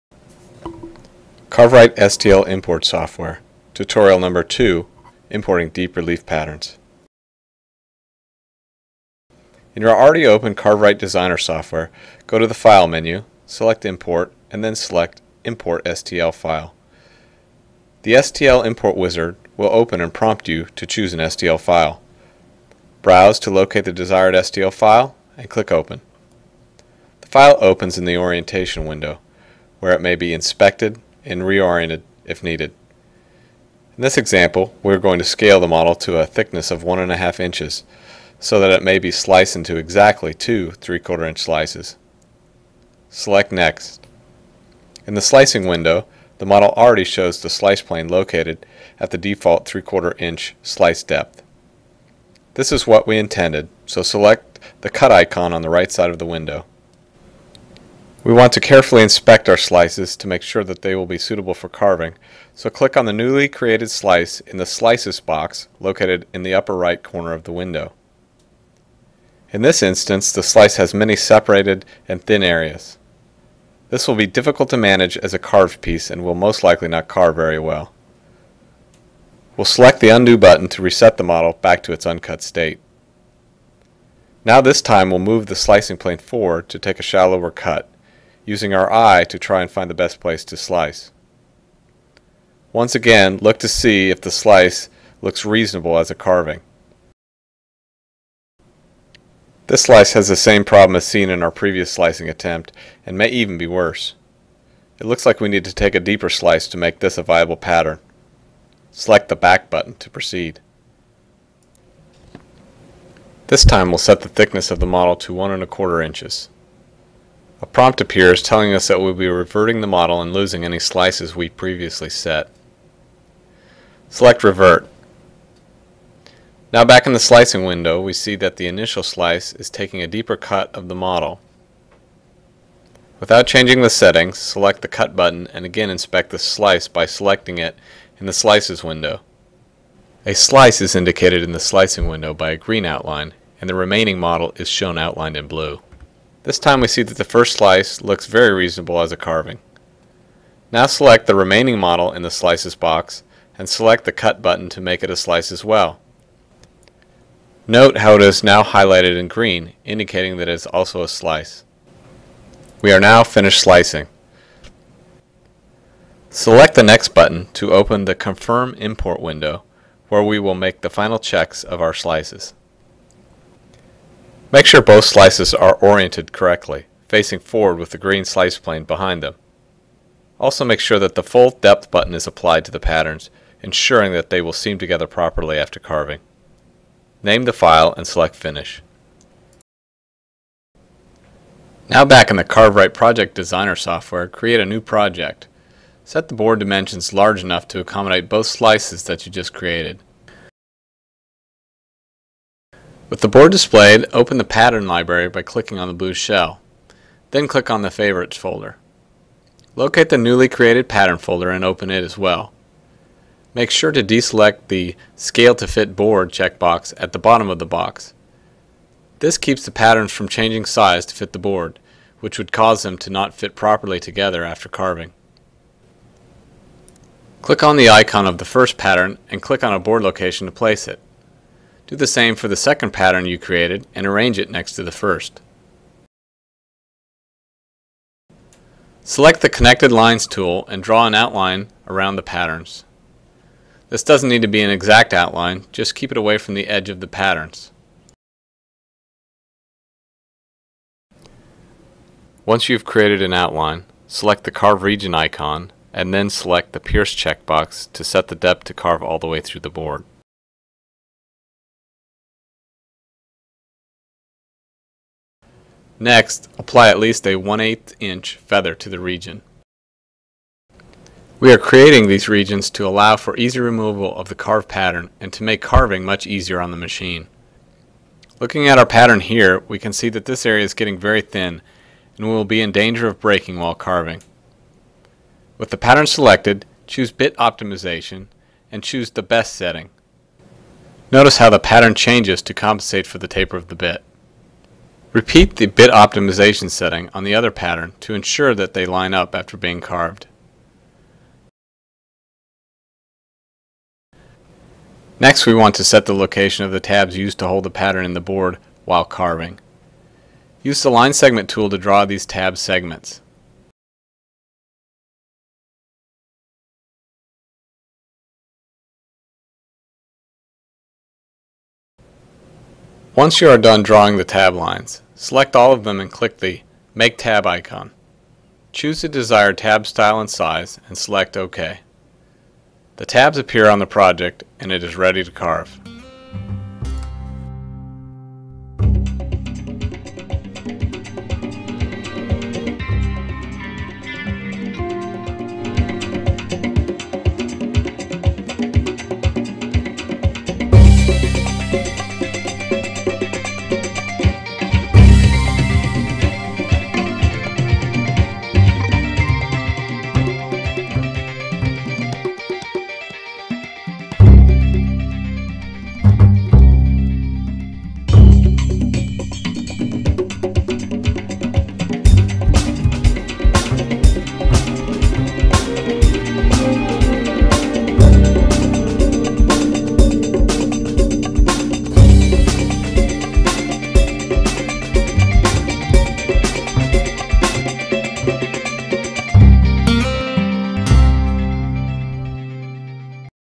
CarveWright - STL tutorial 2
CarveWright's revolutionary carving machine has just become even more revolutionary. The STL import software gives the user the ability to import 3D models as STL's, slice them up for carving and reassemble them into a physical model. It's adding a whole new dimension of possibilities to the CarveWright System. As all CarveWright software, we strive to create the most intuitive and easy to use software solutions on the market. See all the Tutorial video's and see how we are revolutionizing 3D fabrication.